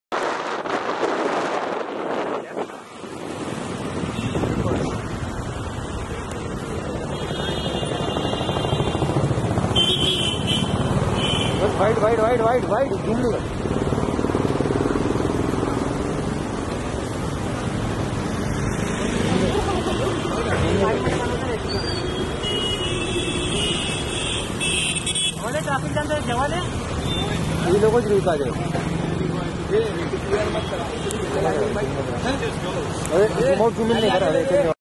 सड़क पर सायकल चलाते Spot Hui Sunny Leone अपने पति Daniel Weber के साथ
sunnyleone